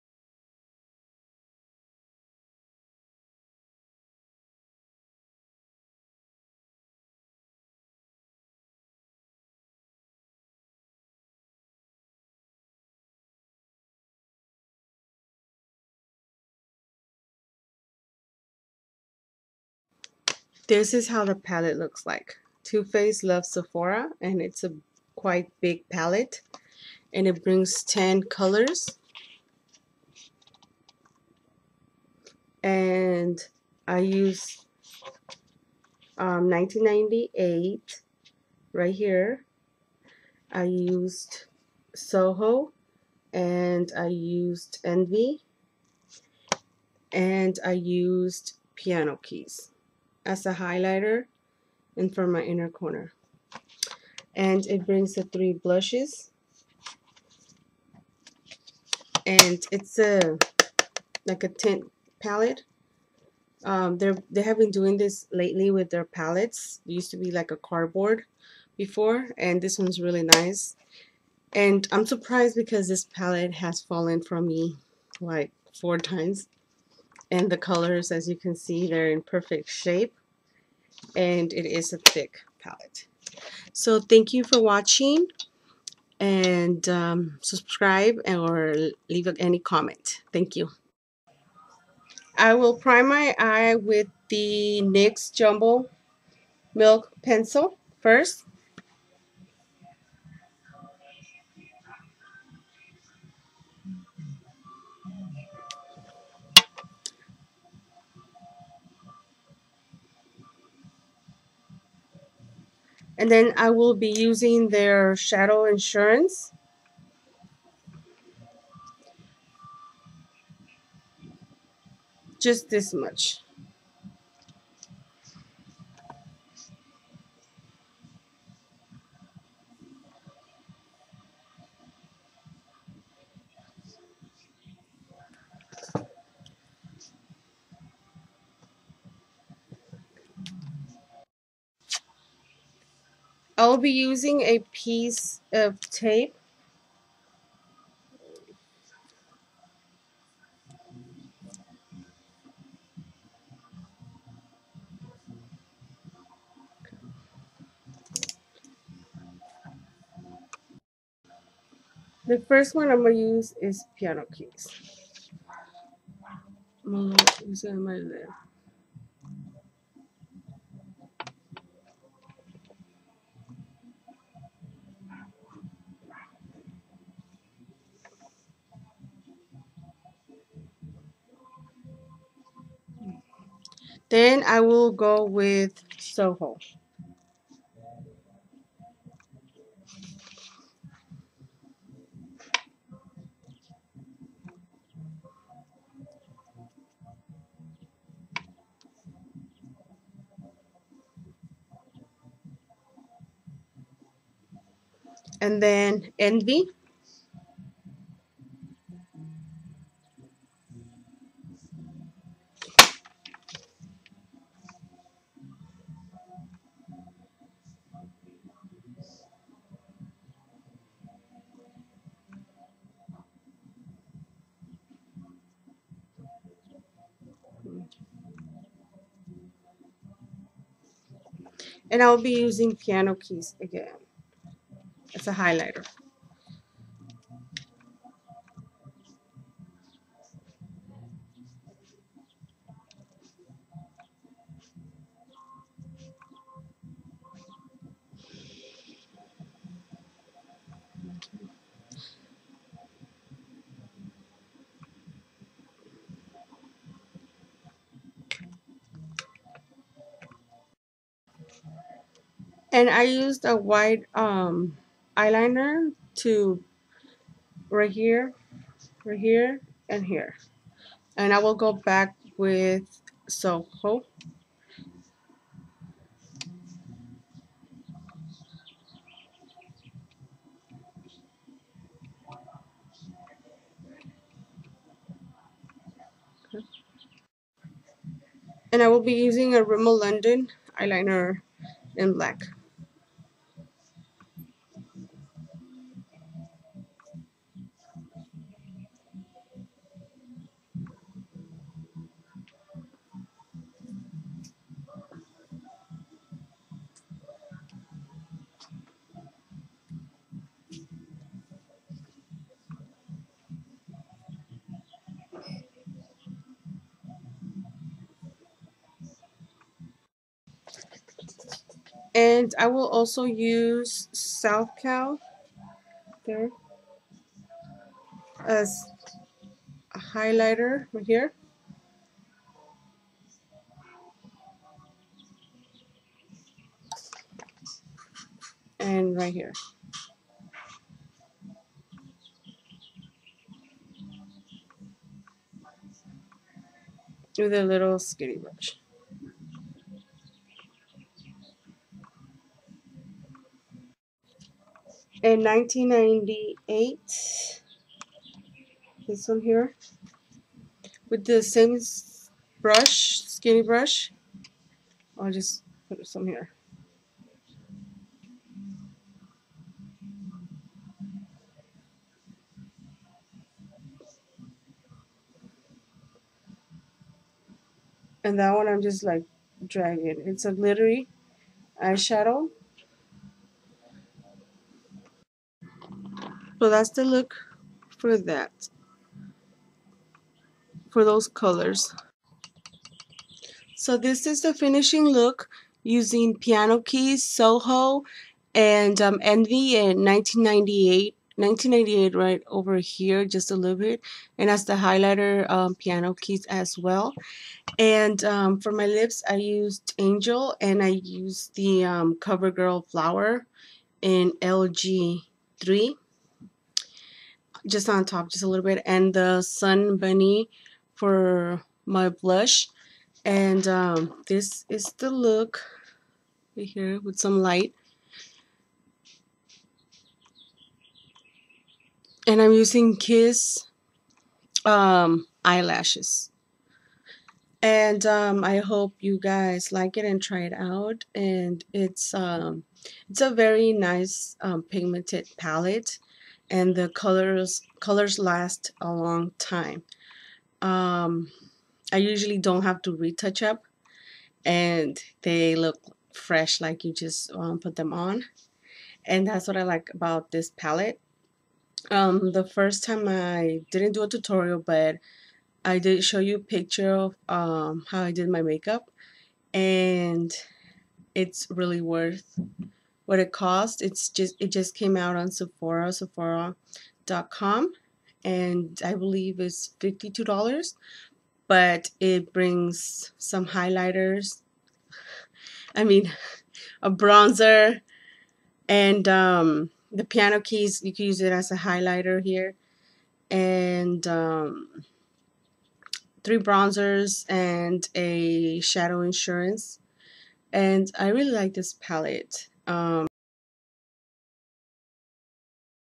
TOO FACED LOVES SEPHORA MAKEUP TUTORIAL
TOO FACED LOVES SEPHORA 15th BIRTHDAY PALETTE TUTORIAL I WILL USE  PIANO KEYS,SOHO, ENVY,1998 SUN BUNNY,SHADOW INS PRIMER.